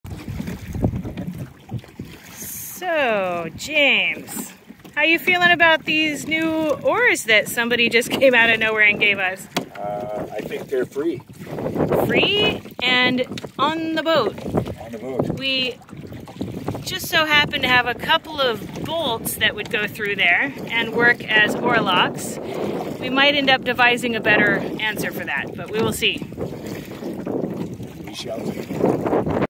A set of oars via the kindness of strangers
We lost an oar in a storm and have been working on building a new one from scrap lumber. Some nice guy came up to James and used only English to say he didn't speak English and then...free! James said okay!

We'll keep looking for longer oars, since length equals power, and real oarlock but this is a brilliant short term answer.